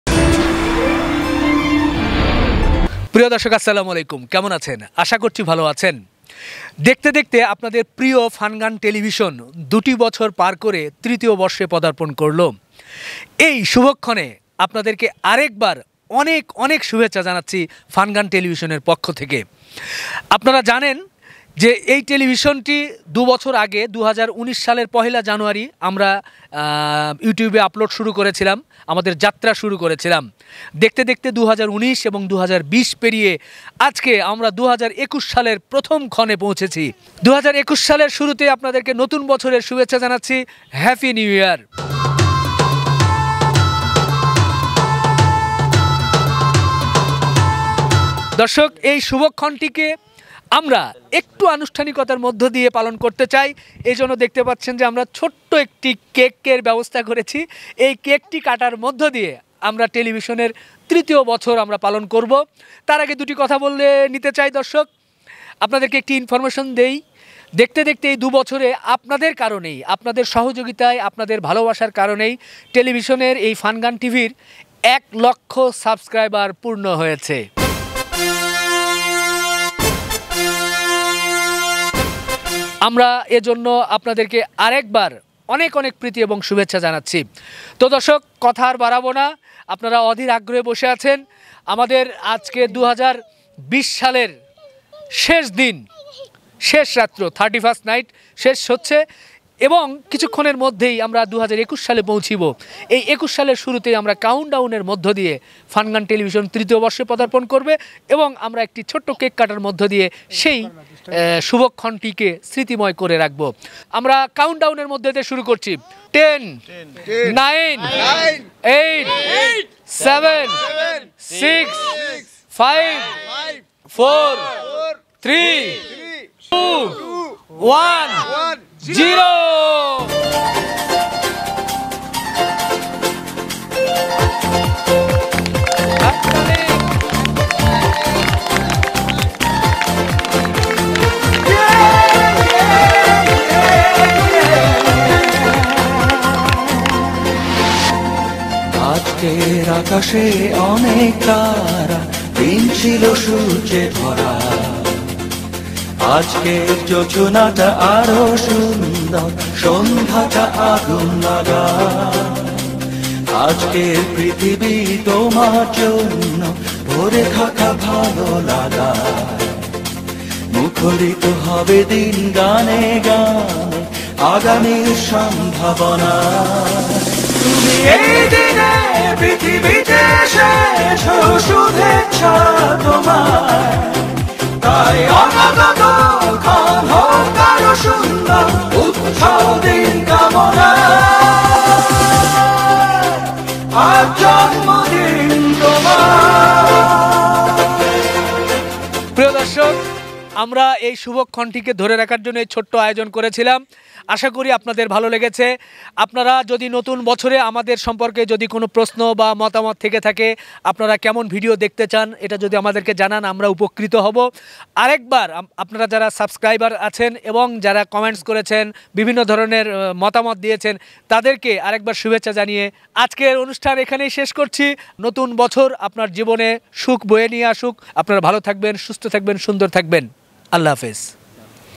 ফানগান টেলিভিশনের ২য় বর্ষপূর্তিতে সবাইকে শুভেচ্ছা । The 2nd anniversary of FUN GAN TV
***  Congratulations and best wishes to all viewers, subscribers, well-wishers and crew on the 2nd anniversary of FUN GAN TV. We are committed to fulfilling your expectations in 2021. I am praying for everyone's advice and blessings for the channel.